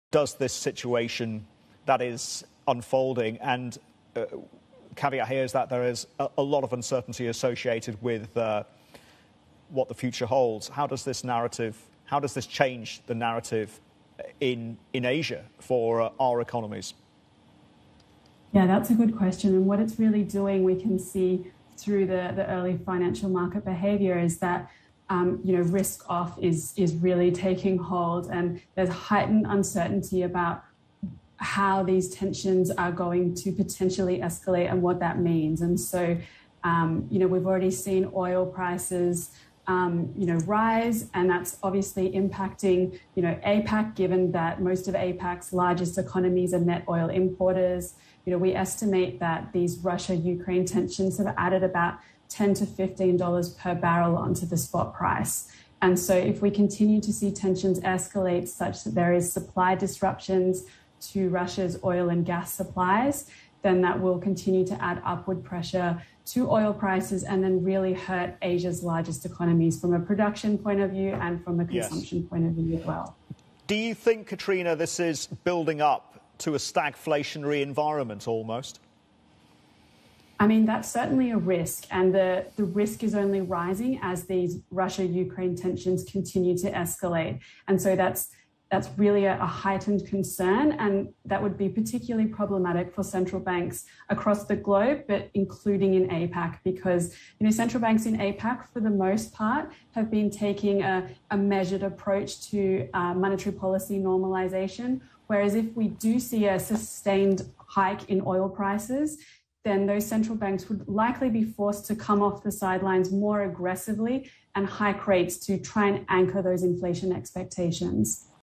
The risk of stagflation is a 'heightened concern' as Russia-Ukraine tensions escalate, says Moody's
Katrina Ell of Moody's Analytics discusses how the Russia-Ukraine crisis might affect oil prices and Asian economies.